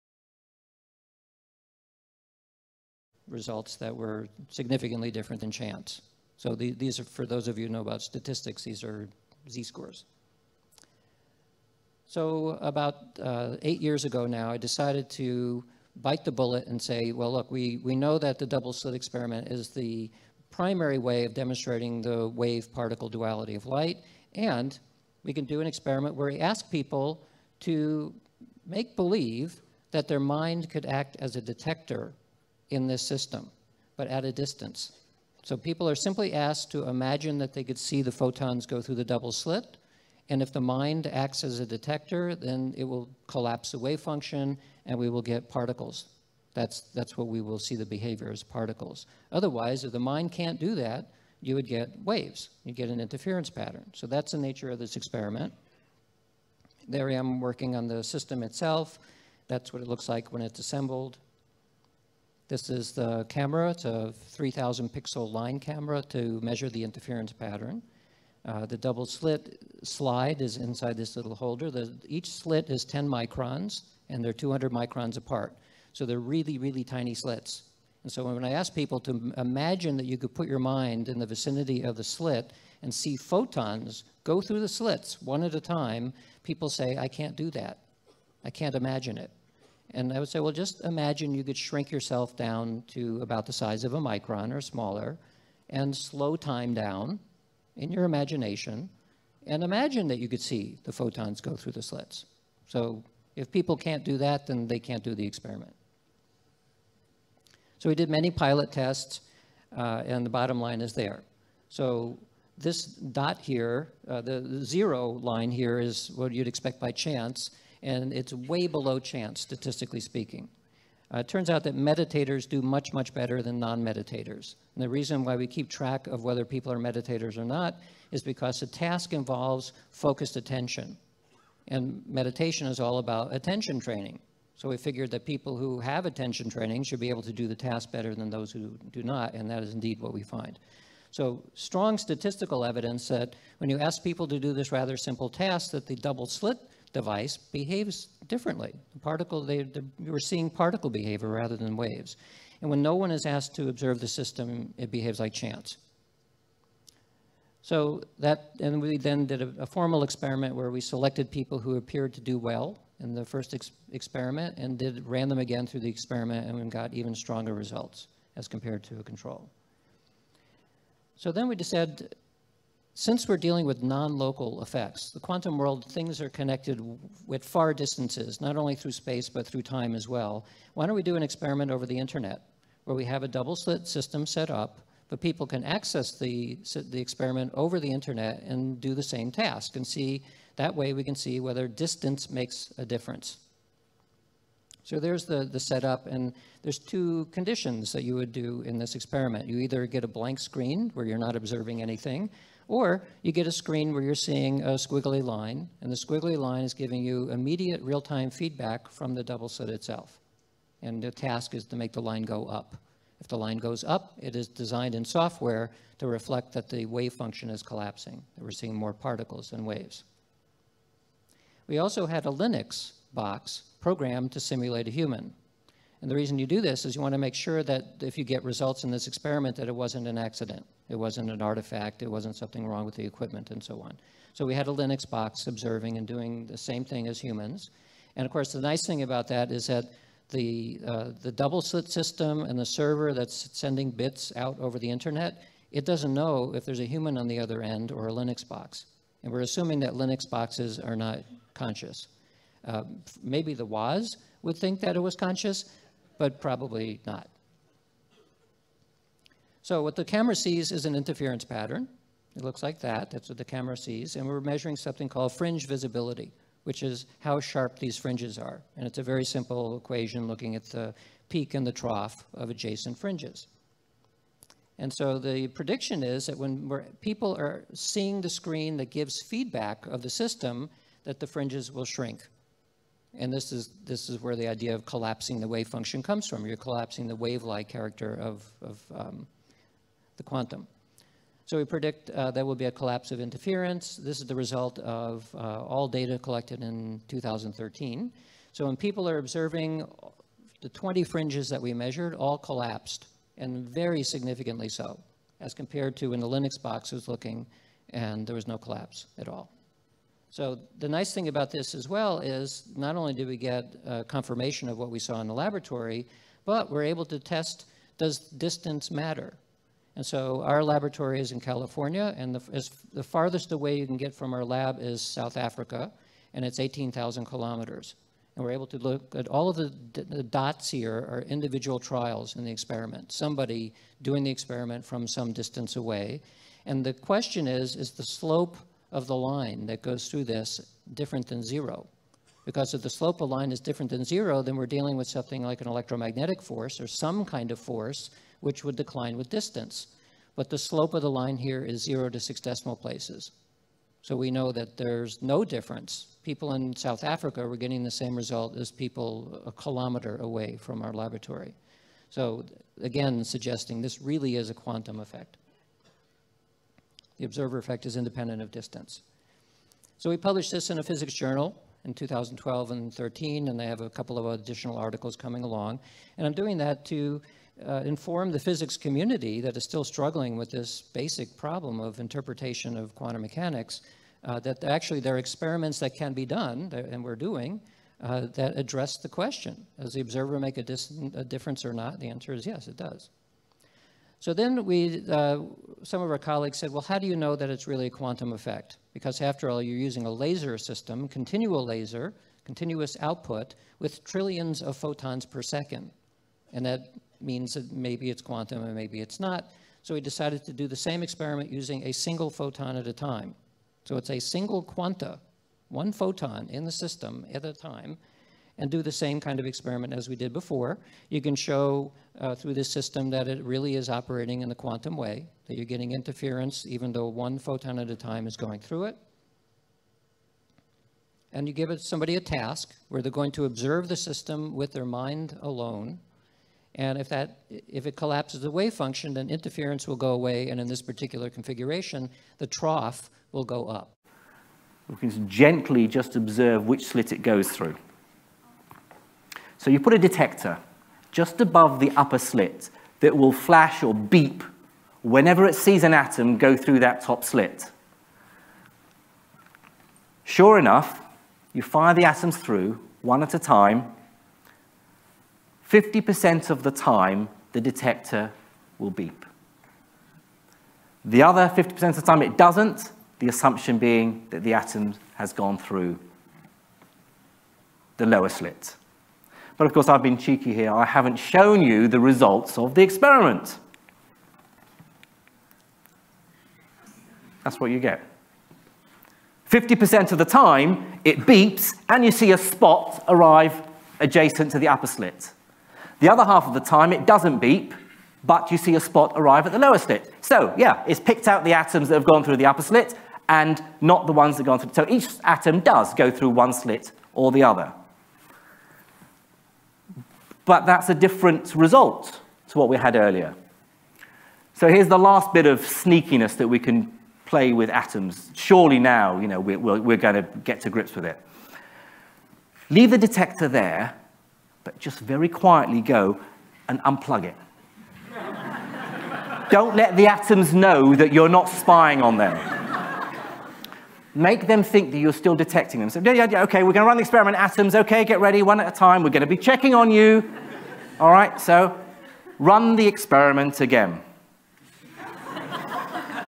Double slit experiment by Dr Dean Radin and Prof Jim Al Khalili of the Royal Society
Two top scientists explain the non intuitiveness of the double slit experiment